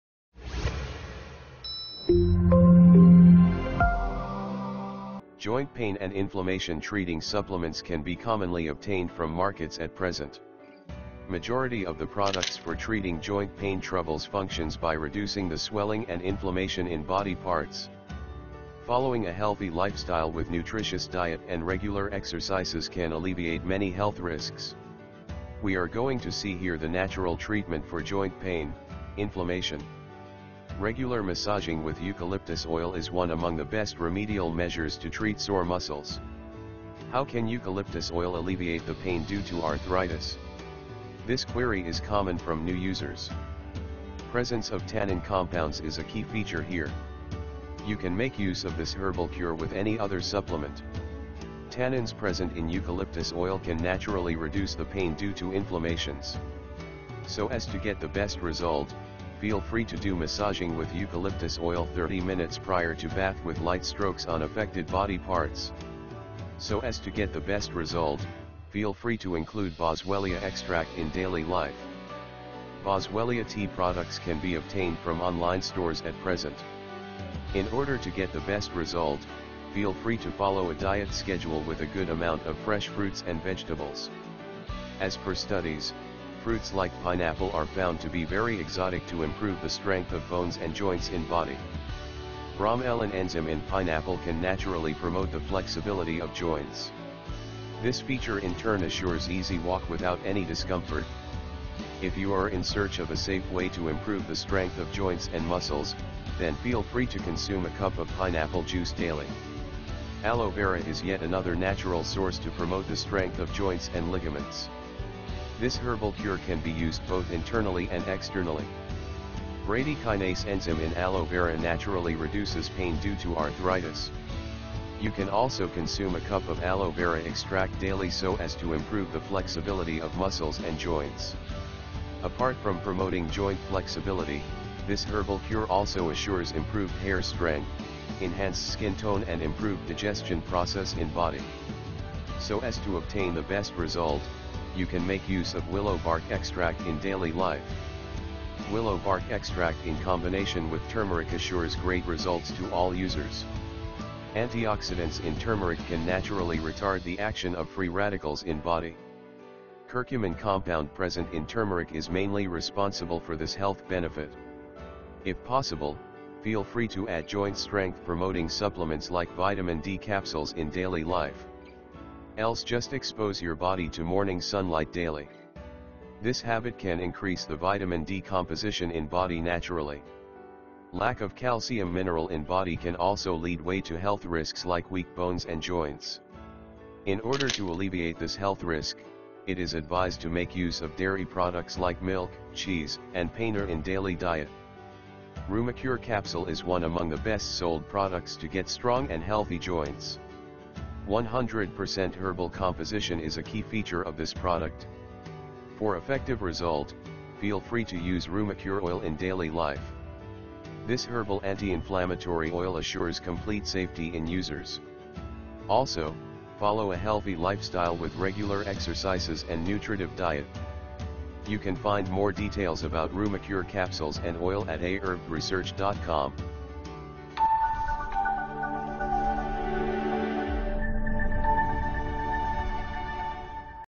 Natural Treatment for Joint Pain, Inflammation
Find the best natural treatment for sore muscles to treat joint pain and inflammation

in this video, we have discussed the best natural treatment for sore muscles to treat joint pain and inflammation. Ayurveda herbs are effective in addressing the root cause of these problems and help to strengthen bones and joints.

Rumacure capsules and oil are purely herbal plant-based preparations that are capable of solving all arthritis and joint-related problems. You can get these pills and oil from reputed online herbal stores.